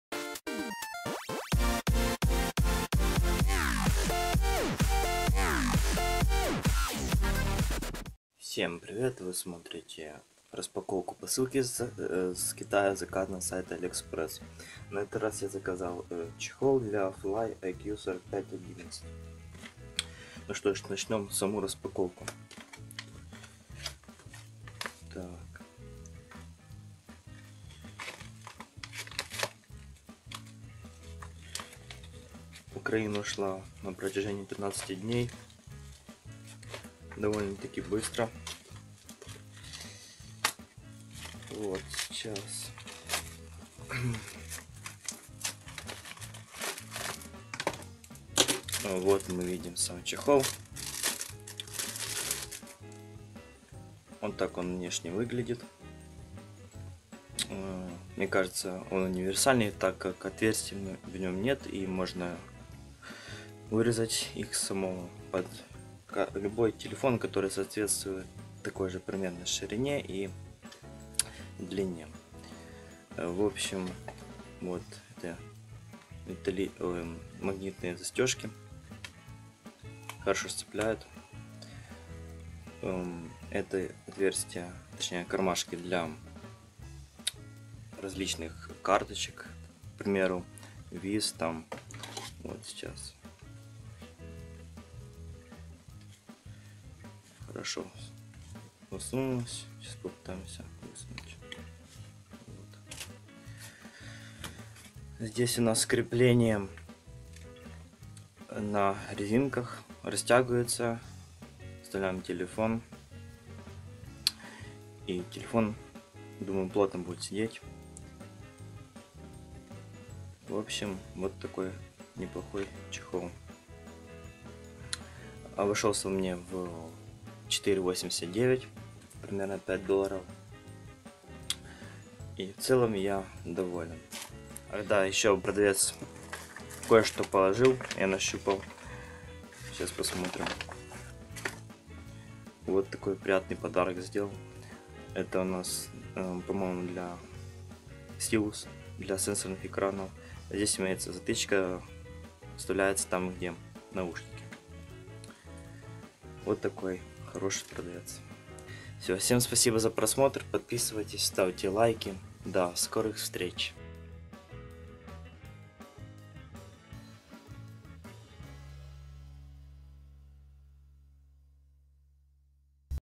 Флип-чехол case для Fly iq4511 заказан с Китая сайт aliexpress Посылка №7
Посылка с Aliexpress. Флип-чехол для Fly iq4511 Посылка #7